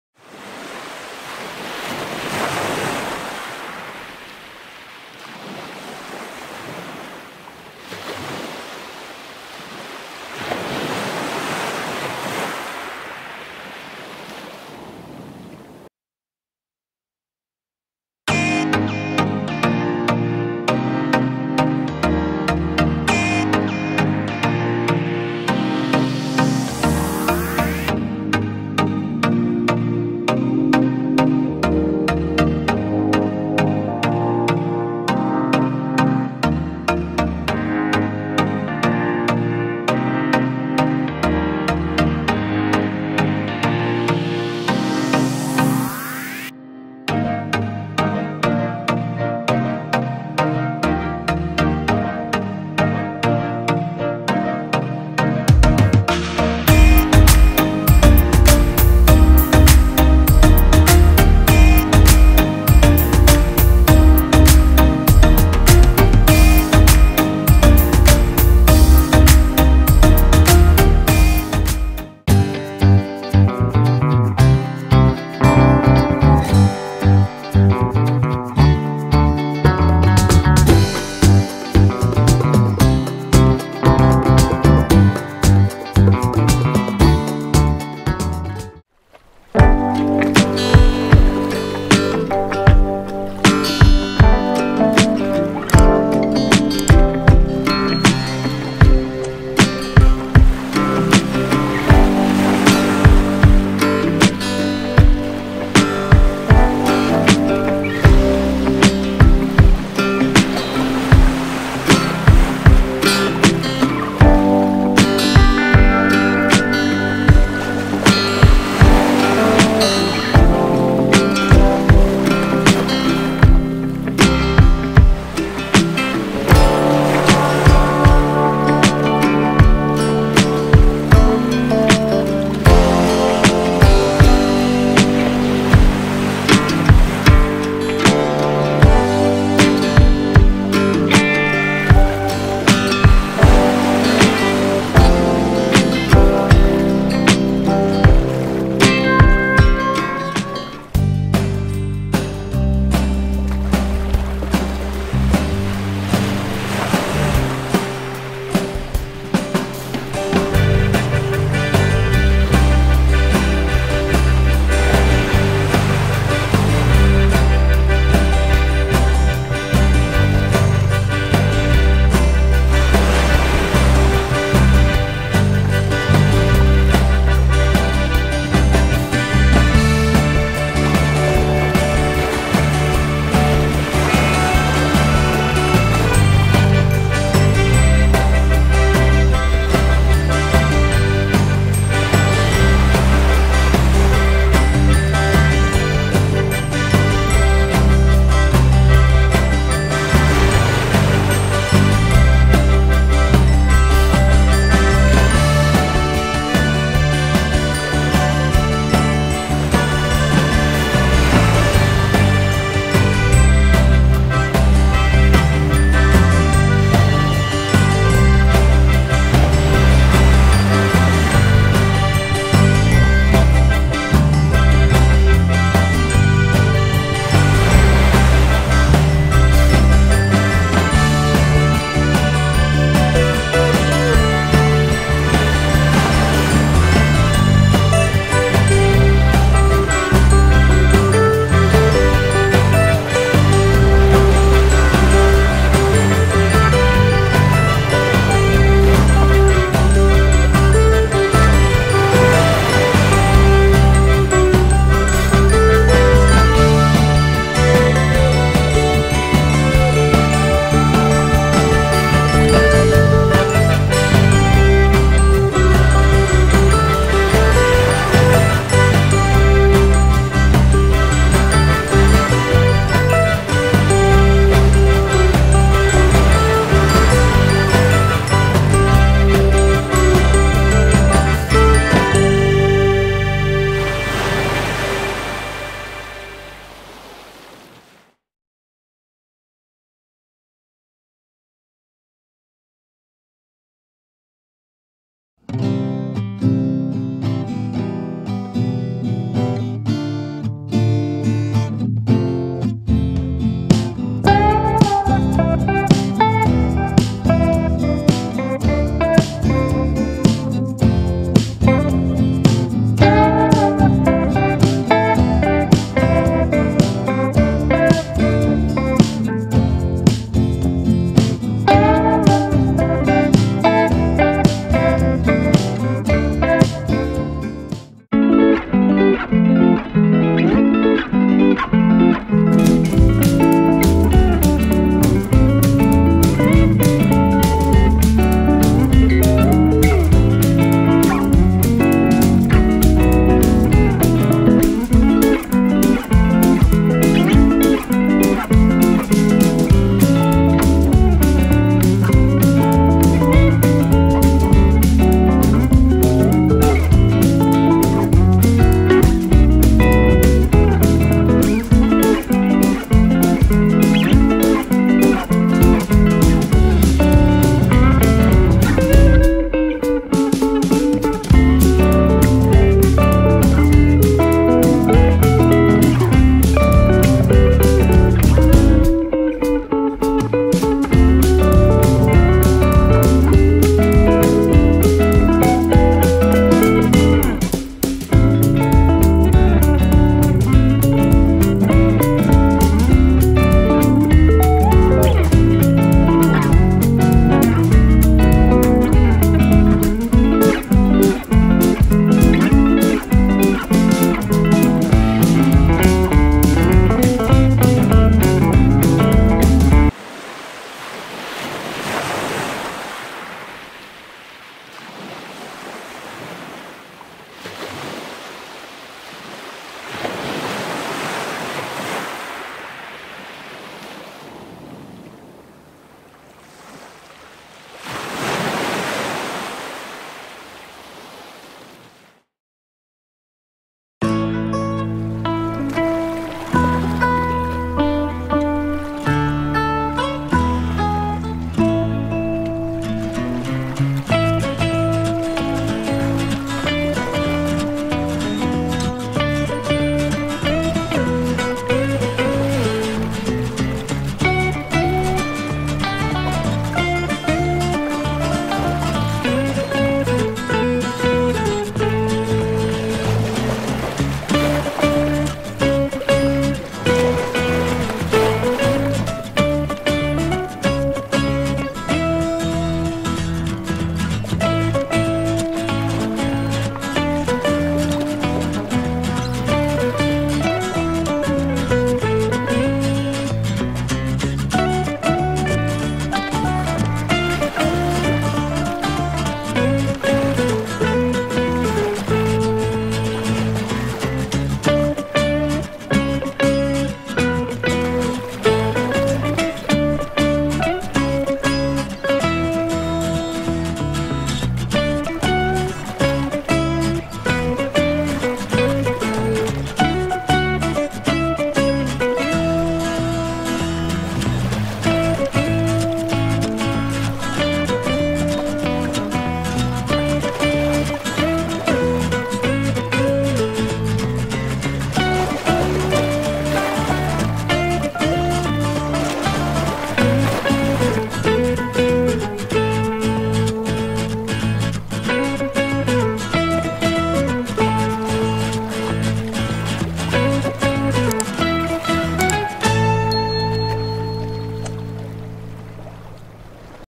FunPamilya TV goes to Liwliwa, San Felipe, Zambales [4K]
Zambales is known for its popular beaches.
And one of them is Liwliwa beach.
Located in San Felipe, Zambales, Liwliwa is a prime surfing spot three hours away from Manila.
For this trip, we stayed at Flora's at Liwa Beach Resort.
Their rate is affordable, and they do have good amenities.